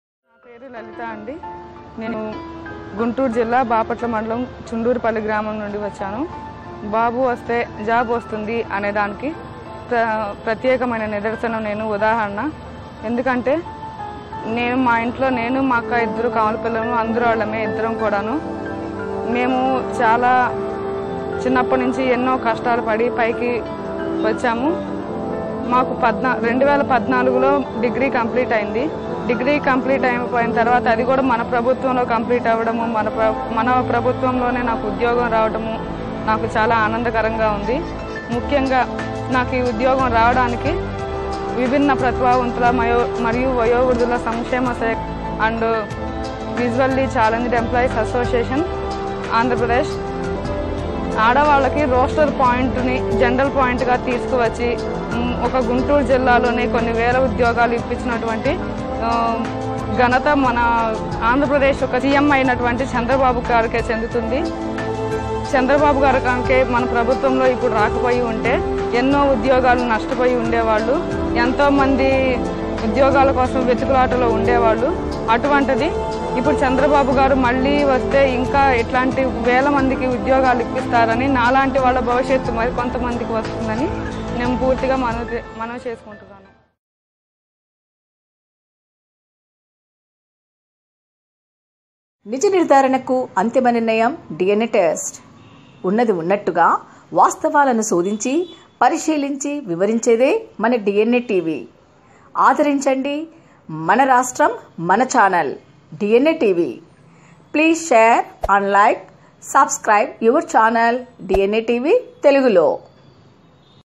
నేను అంధురాలిని నాకు జాబ్ వచ్చింది..జాబు కావాలంటే బాబు రావాలి | DNA Telugu TV
జాబు కావాలంటే బాబు రావాలి | DNA Telugu TV